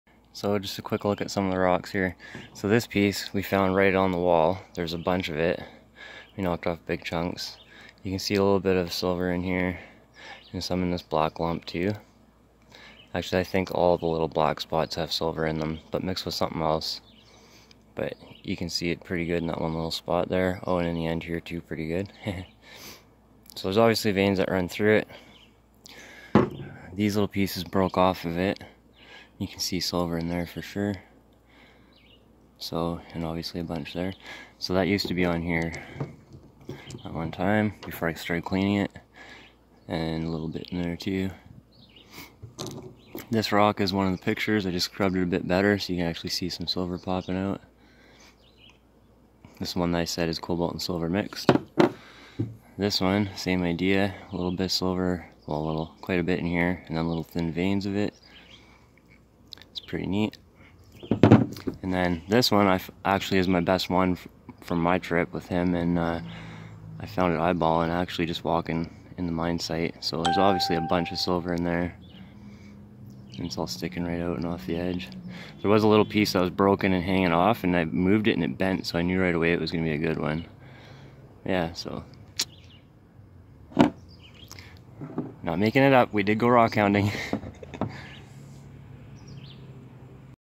Silver ore I found exploring with Aquachigger! (A couple months ago)
Just a quick look at some silver bearing ore I found exploring a mine with Aquachigger!!